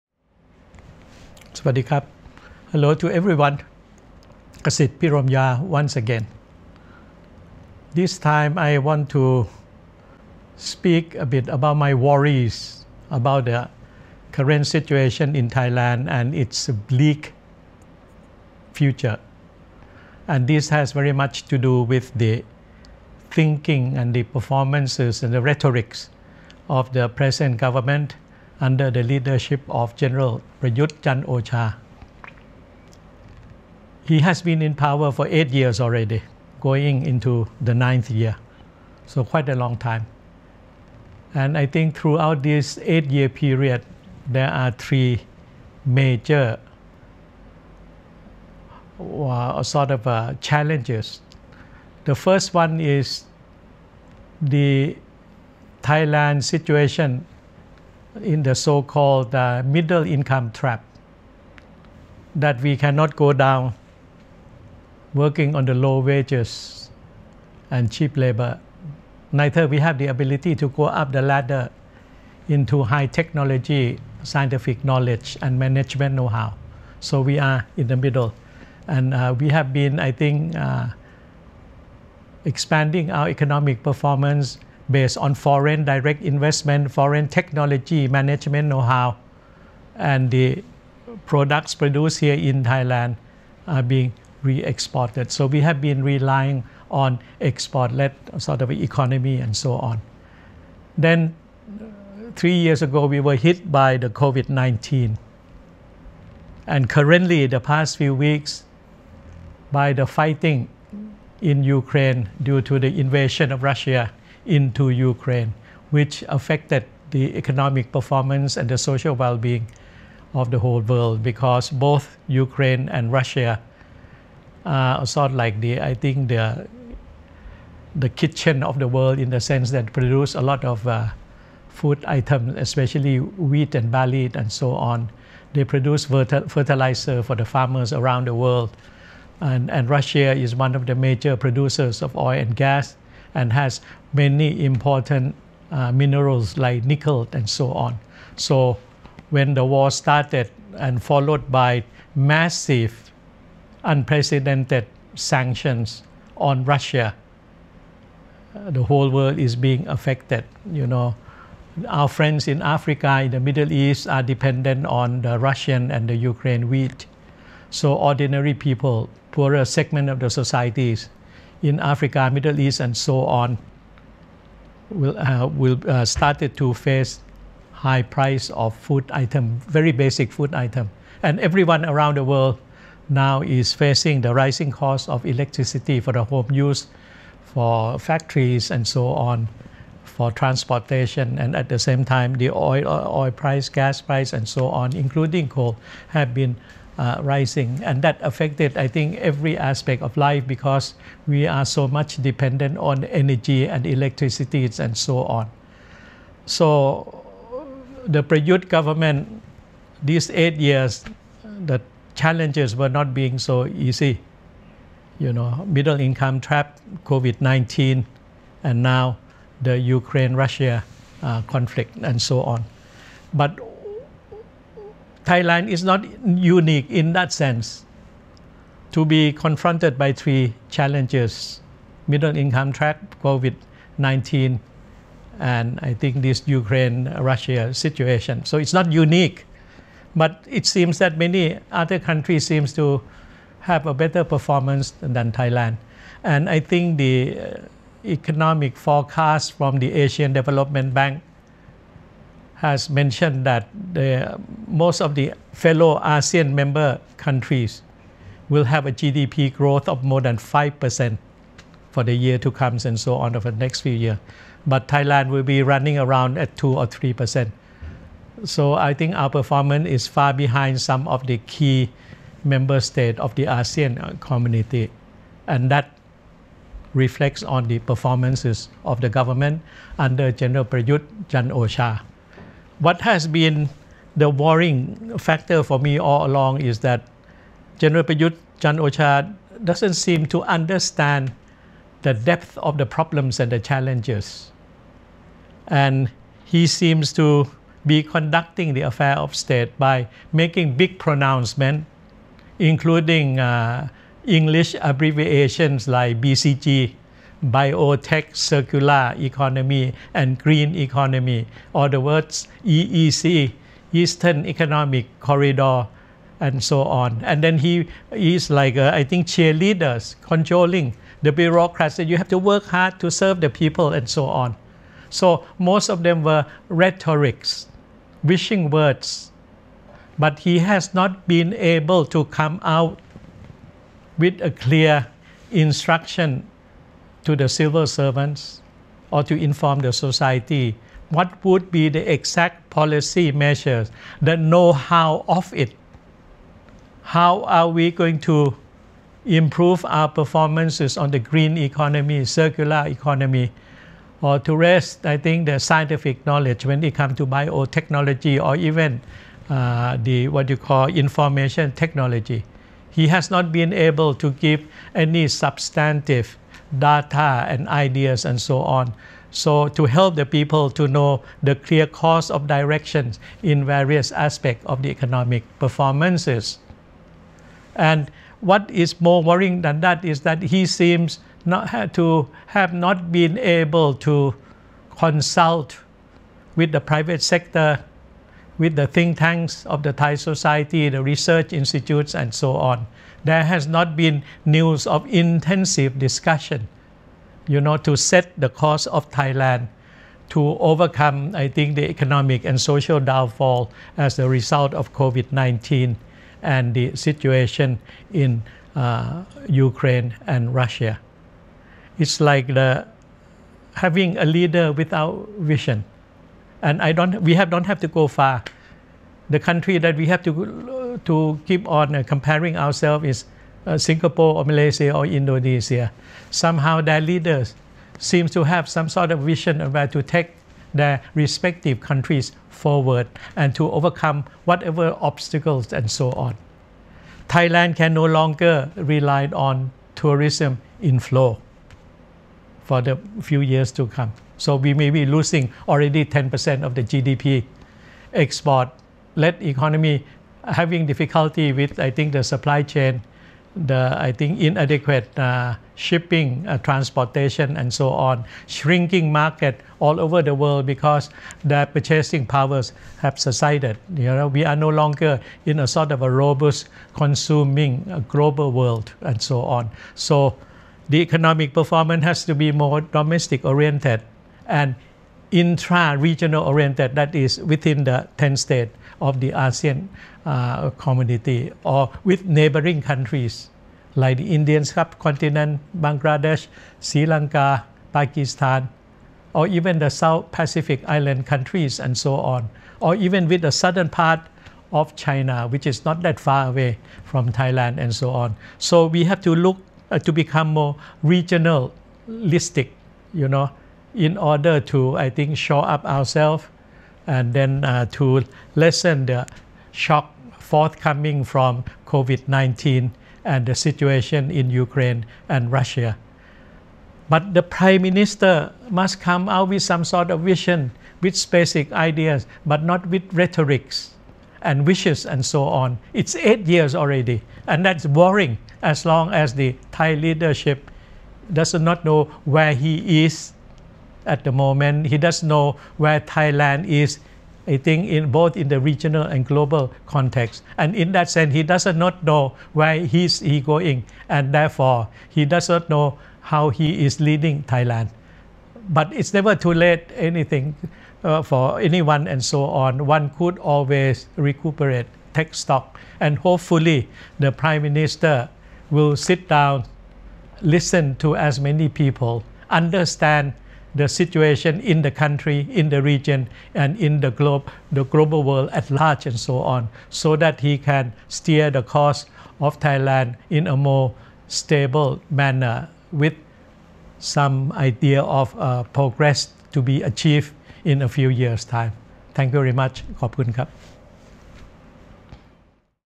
Worries About Thailand | Kasit Piromya (09-04-22)
Thailand: lots of policies vs not a lot of leadership, abundant resources vs not abundant 21st century skills. What are your concerns and ideas?